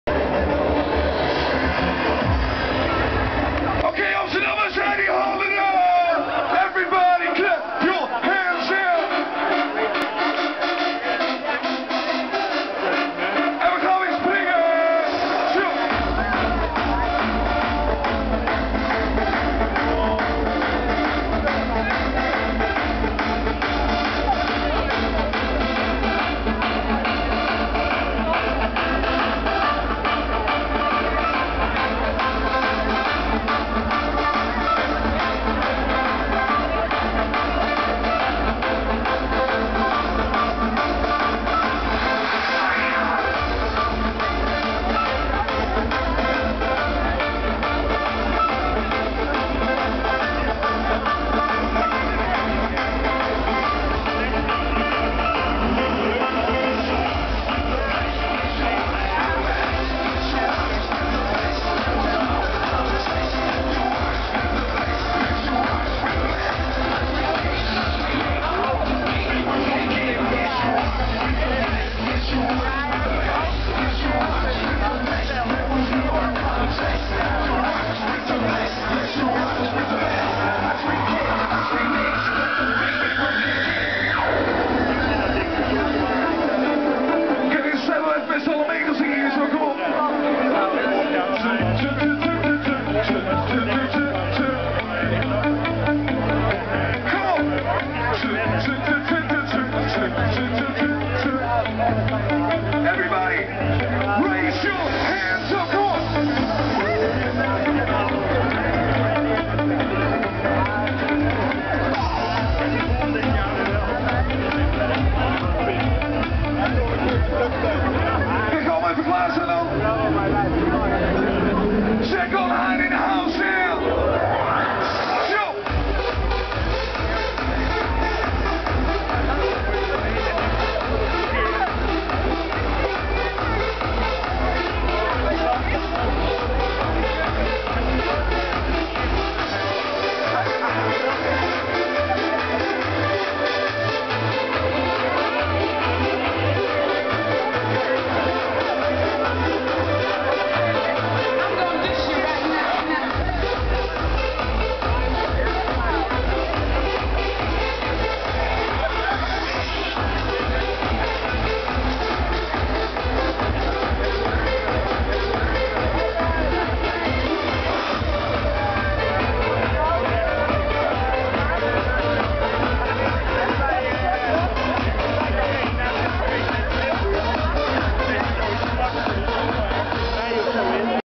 Amsterdam Uitmarkt 2007 - Jeckyll and Hyde
A view on the performance of Jeckyll and Hyde at Amsterdam's Uitmarkt of 2007. Jeckyll and Hyde are candidates for a nomination for the upcoming MTV European Music Awards.